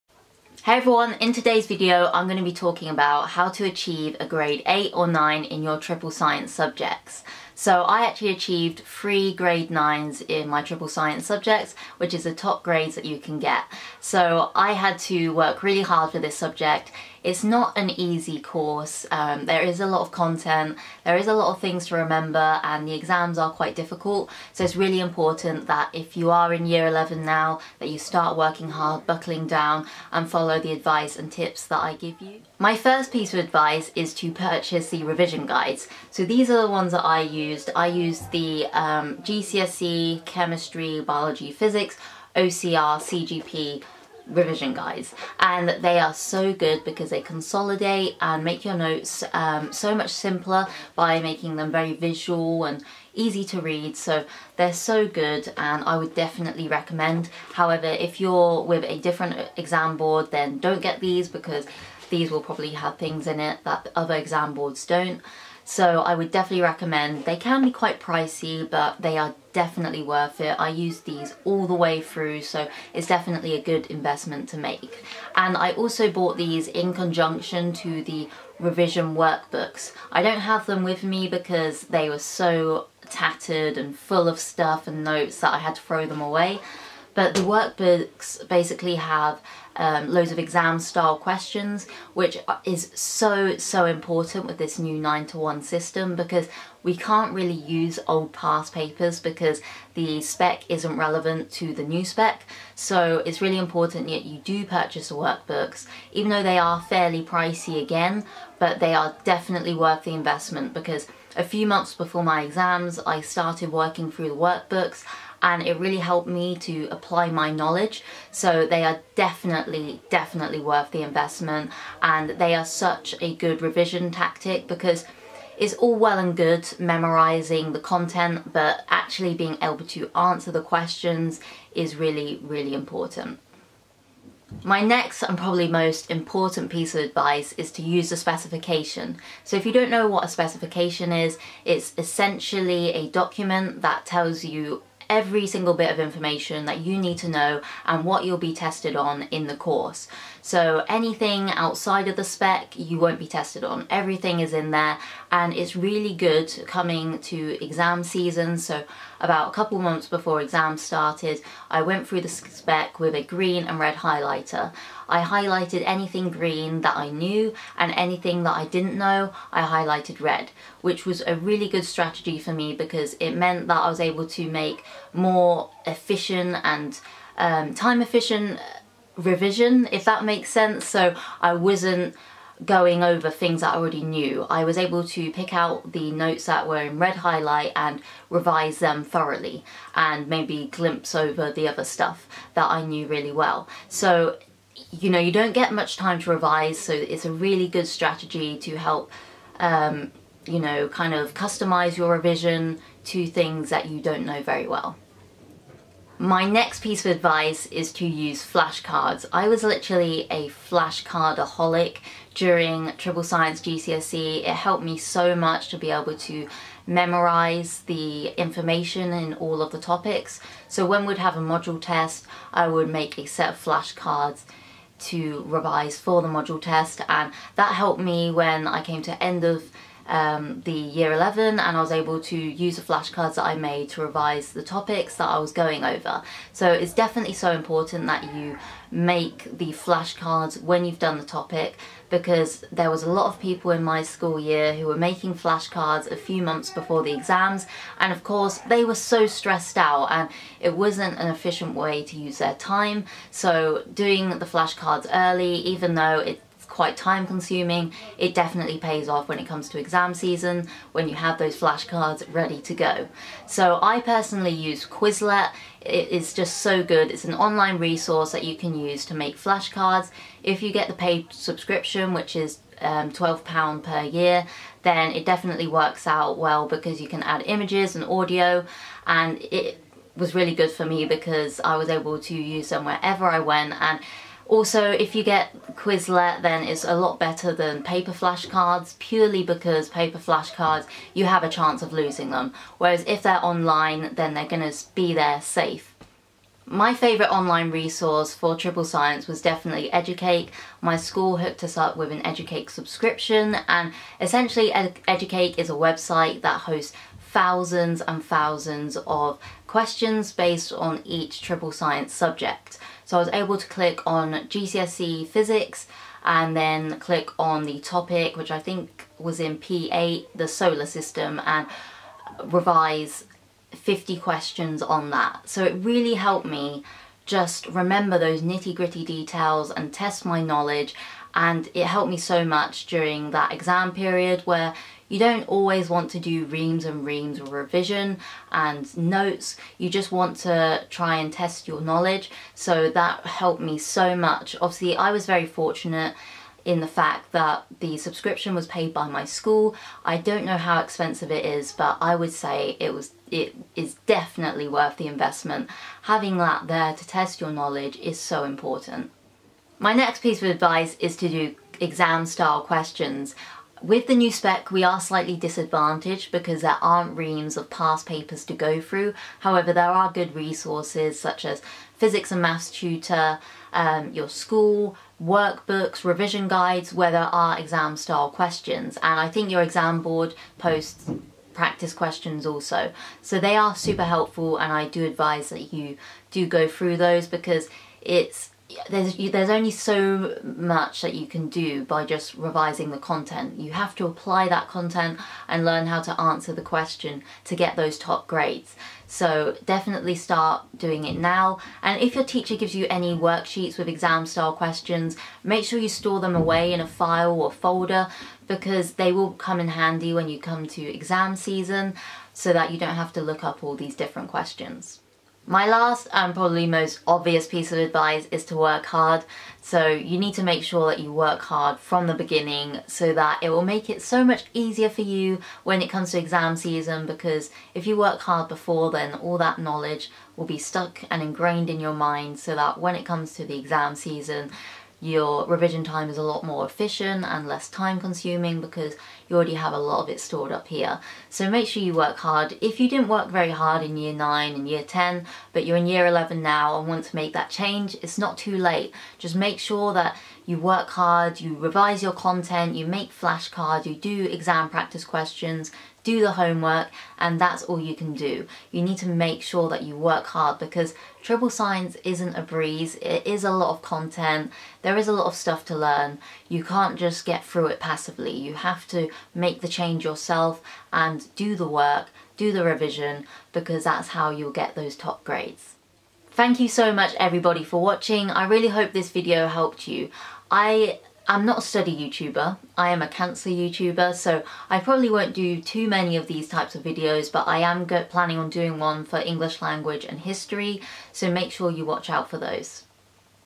How to get a grade 8 or 9 in Triple science | Revision strategies + tips!!!
I hope my advice helps!
Ellie xxx